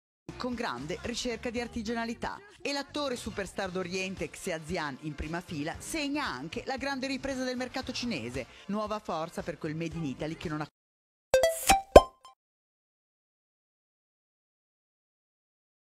230225#肖战上意大利电视了#肖战上意大利电视啦！意大利节目称来自亚洲巨星的肖战在品牌活动期间坐在了第一排，这凸显了意大利制造在中国市场的逐渐上升。国际影响力max
肖战米兰第一场看秀路透+官方镜头合集（15分钟）怼脸镜头好帅，笑容治愈，大明星游刃有余#肖战米兰街拍